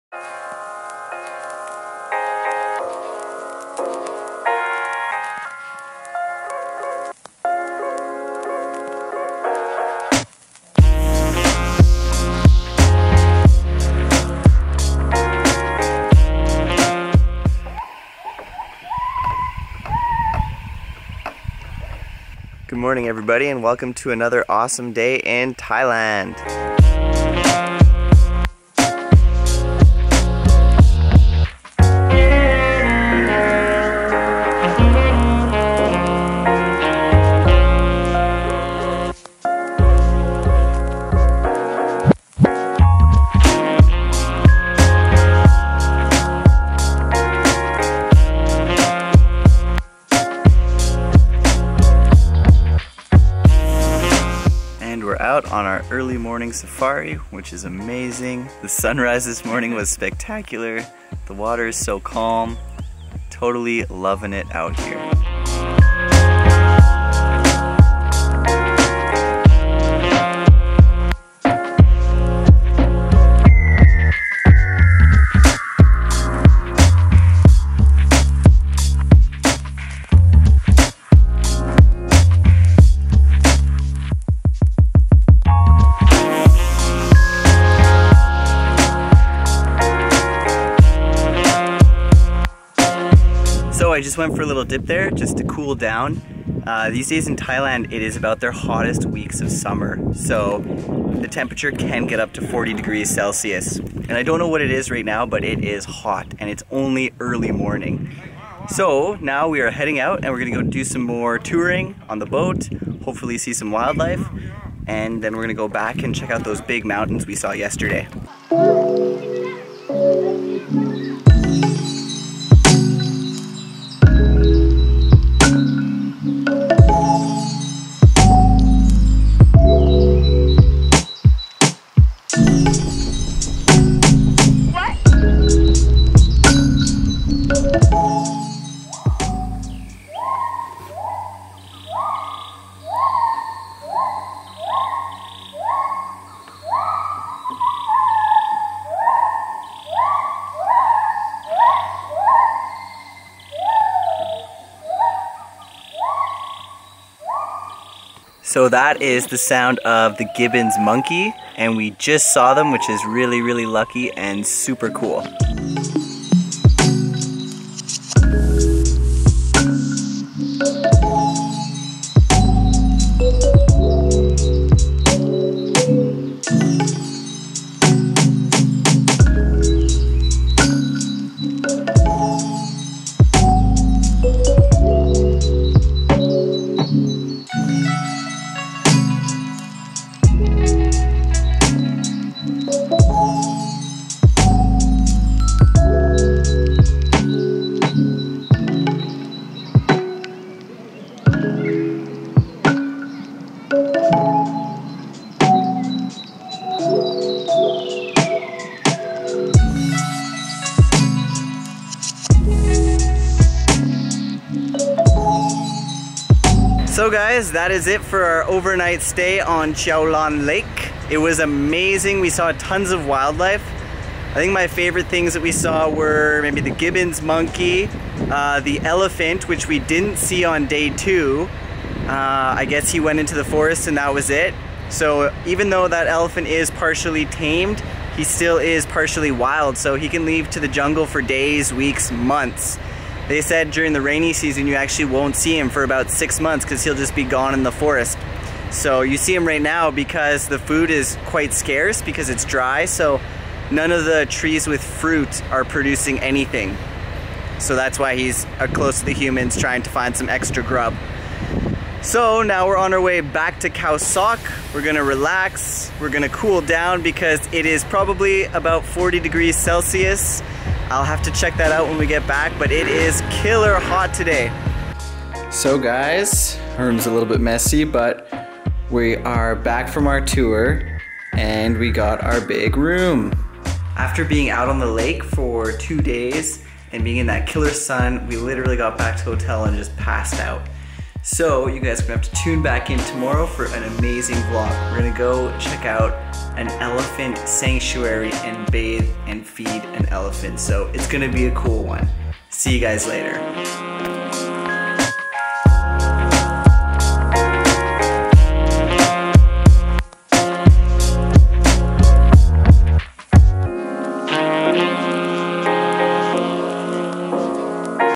Raft House in Thailand
Thailand Travel Blog  // Day 13  // Zokah Photo & Video
Raft House Thailand - Amazing Adventures in Thailand

We're exploring Thailand as a couple and we can't wait to see what this county has to offer! Follow us for more adventures from Thailand.

Keep your eyes open for more VLOGS!

All filmed on the Sony RX100 Mark 5, 5dm3, DJI Spark